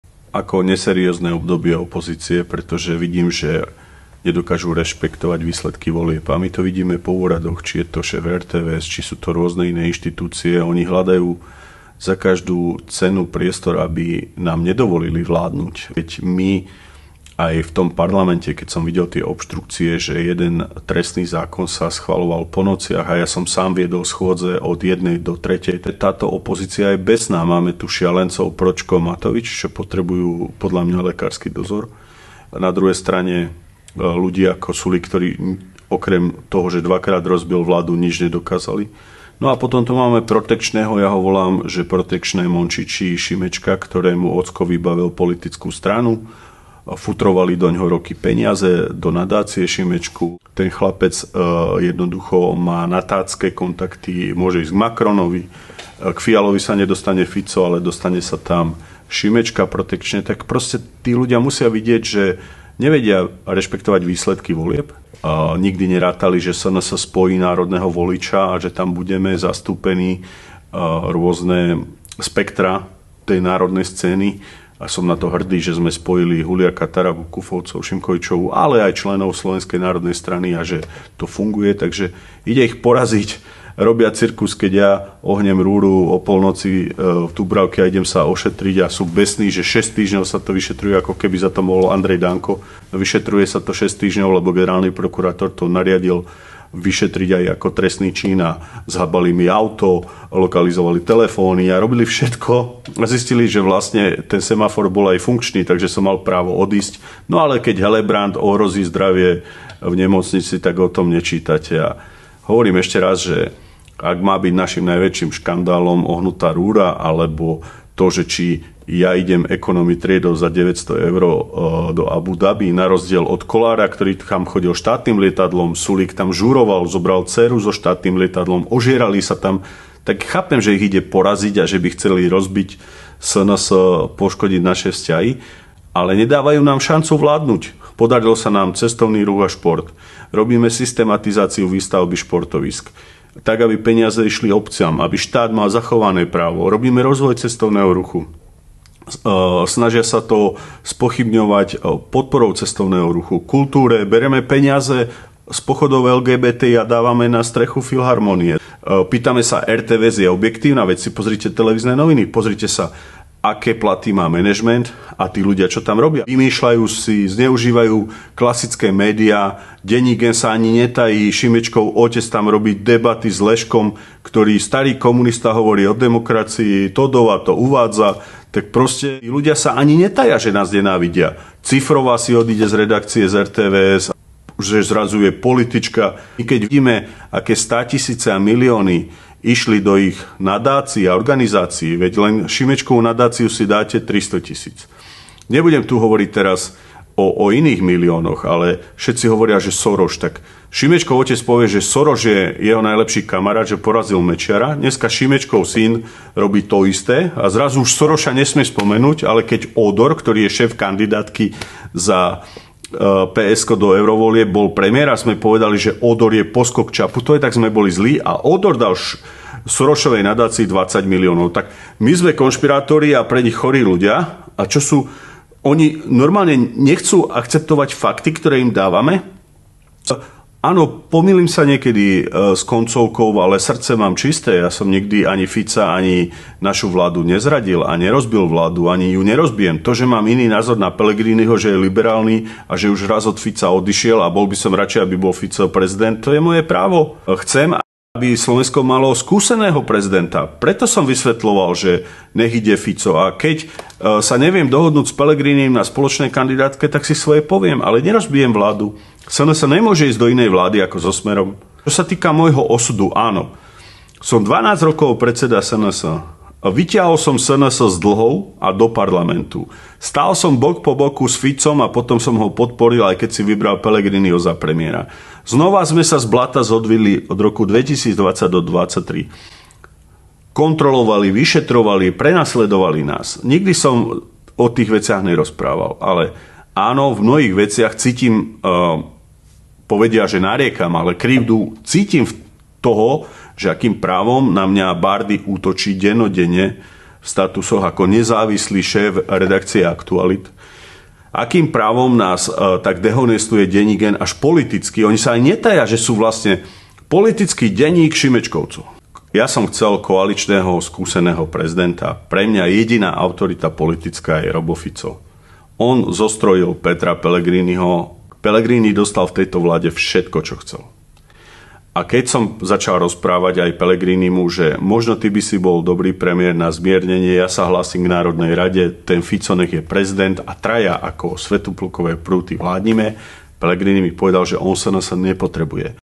Srdce mám čisté a chcem, aby Slovensko malo skúseného prezidenta!
Liberáli by chceli rozbiť SNS, poškodiť naše vzťahy. Všímajú si semafor, ale to, ako sme podporili cestovný ruch, šport, výstavbu športovísk a kultúru na Slovensku, to nie. Tvoríme hodnotové veci a máme i hmatateľné výsledky.

Liberáli ma nemajú radi, pretože mám tvrdú ruku a názory, za ktorými si stojím.

Vnímam, že opozícia zneužíva rôzne skupiny ľudí na to, aby si ich manipulatívne získali ako voličov. Chcú kaziť meno tým, ktorí chcú na Slovensku zaviesť poriadok.